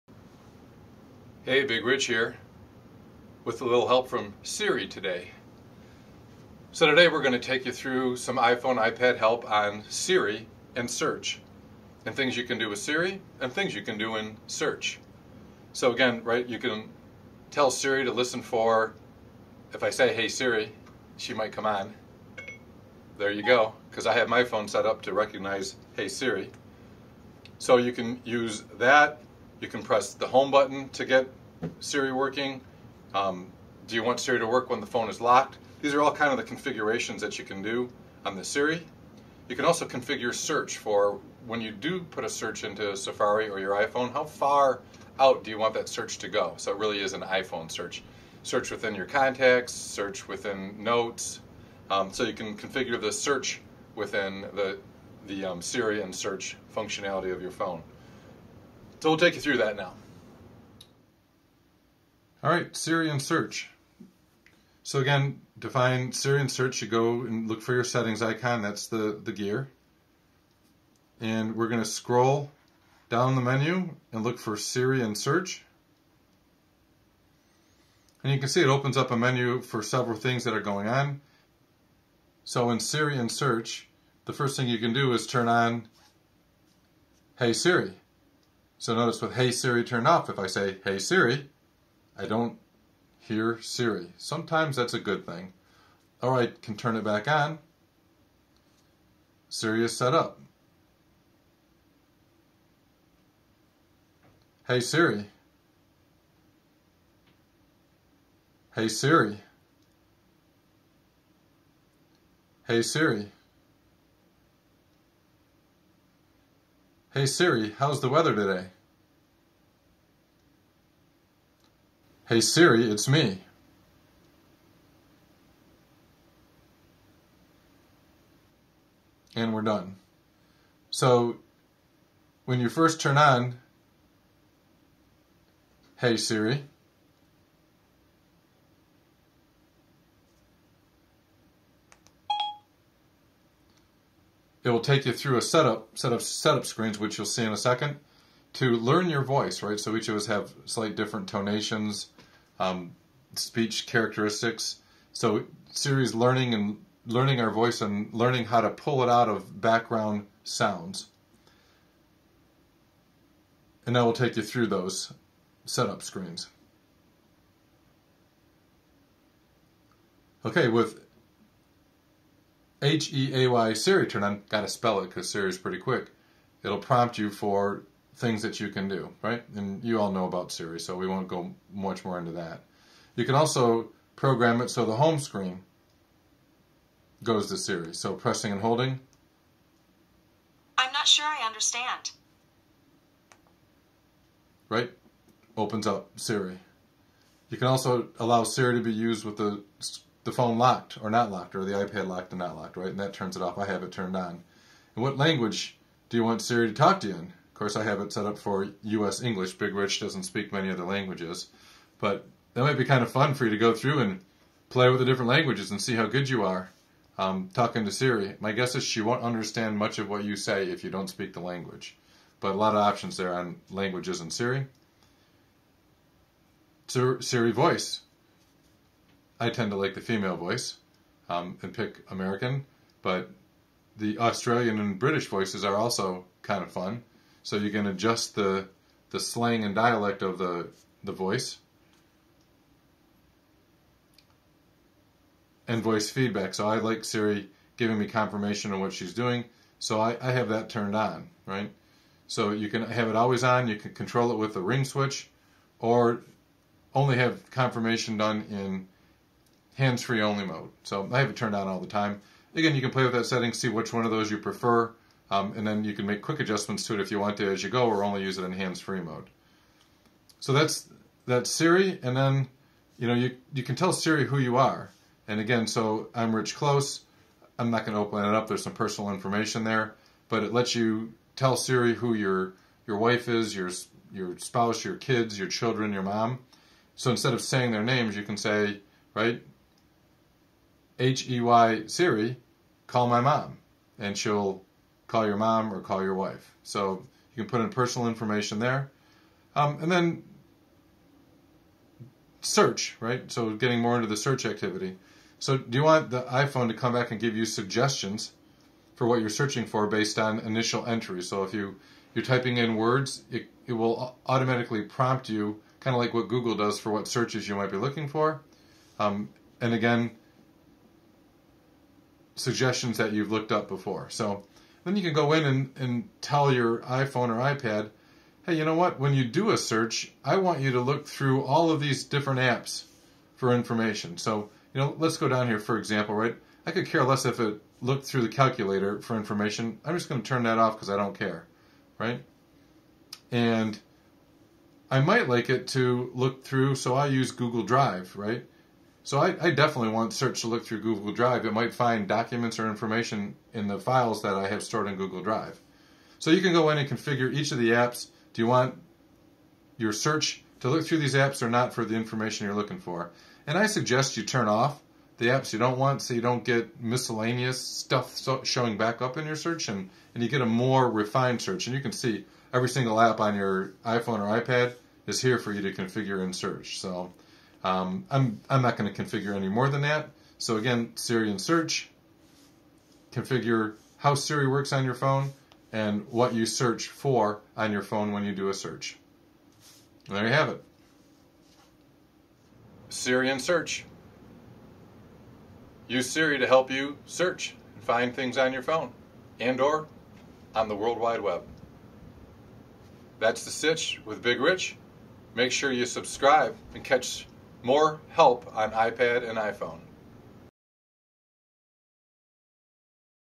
iPhone Siri and Search Settings with Big Rich Close
Big Rich takes you through the Siri and Search Settings menu for iPhone or iPad.  Configure Siri for how you want her (or him) to work. Customize Siri to recognize common names for your wife, husband, children or parents.